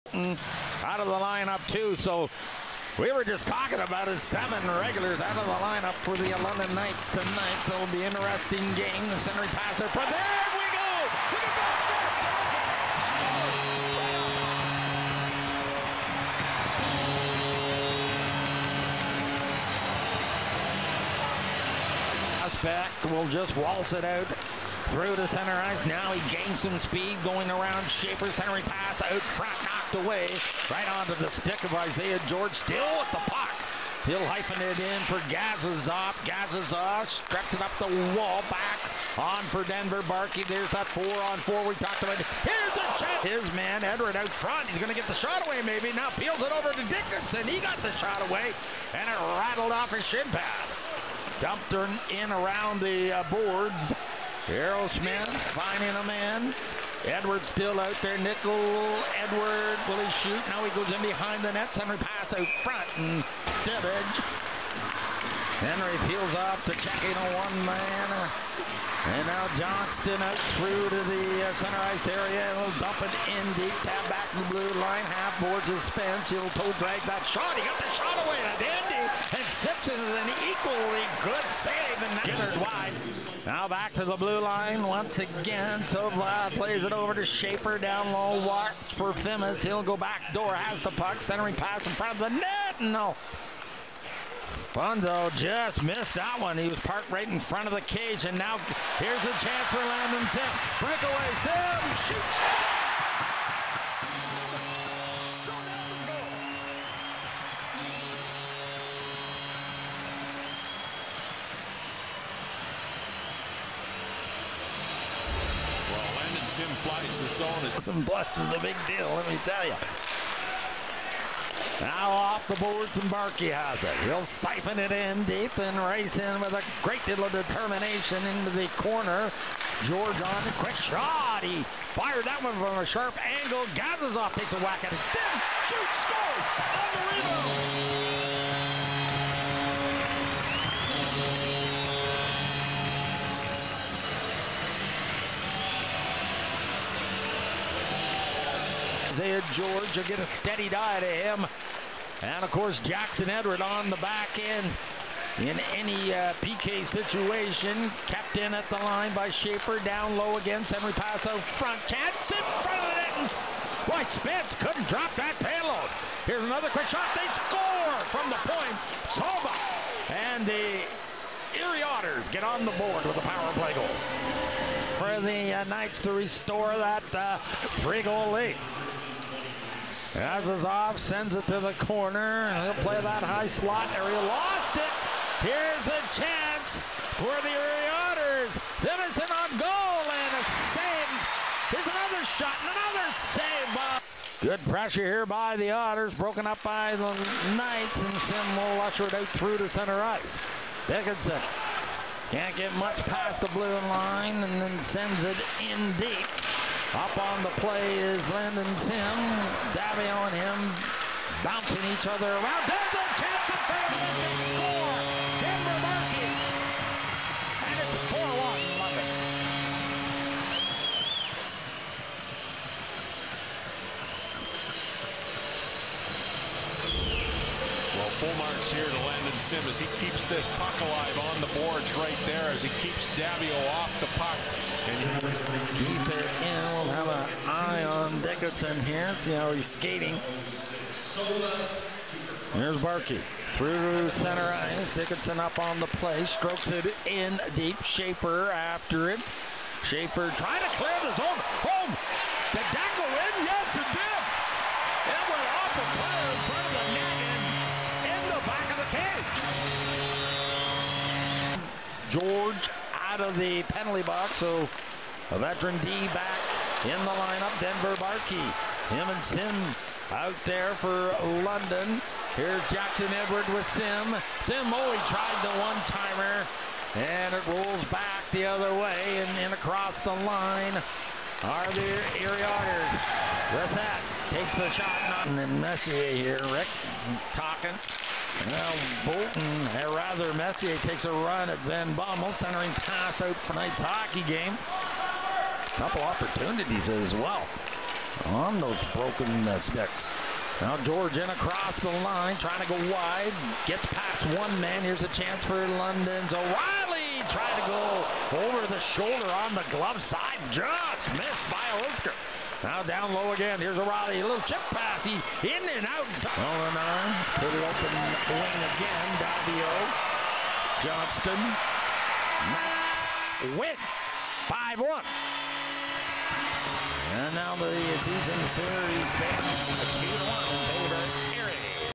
Otters @ Knights 01/03 | OHL Highlights 2024
The Knights opened 2024 extending their win streak to seven games!

Post Game Report Presented By Canada Life